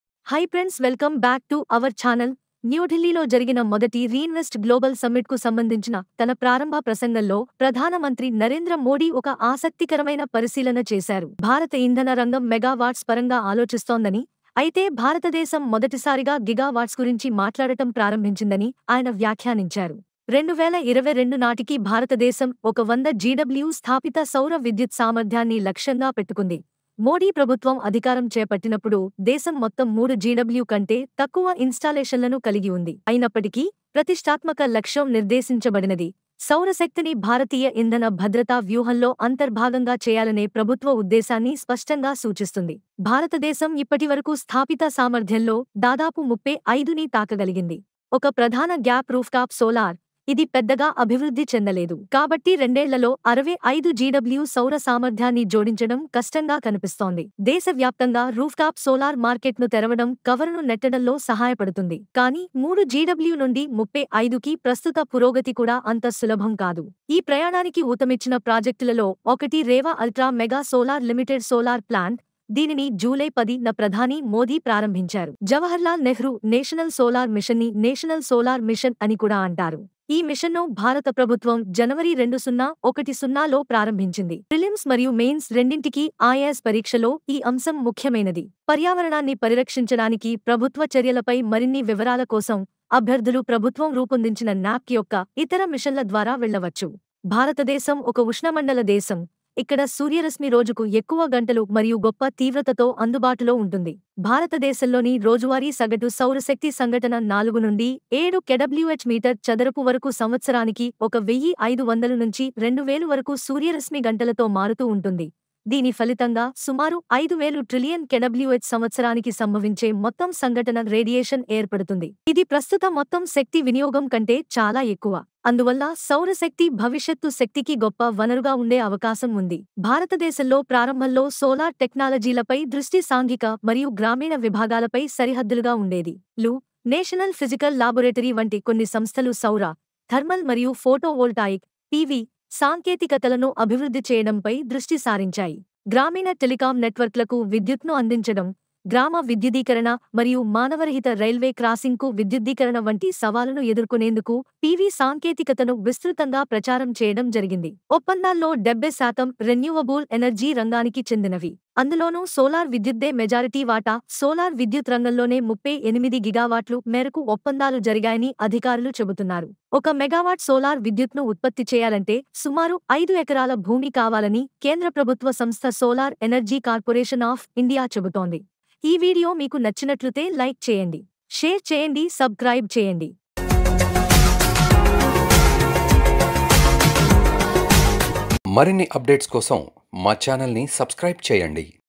సముద్రంలో నుండి బయటకు వచ్చిన 2 లీటర్ల చమరు నూనెతో భారత్ సృష్టించిన సరికొత్త చరిత్ర
watch: సముద్రంలో నుండి బయటకు వచ్చిన 2 లీటర్ల చమరు నూనెతో భారత్ సృష్టించిన సరికొత్త చరిత్ర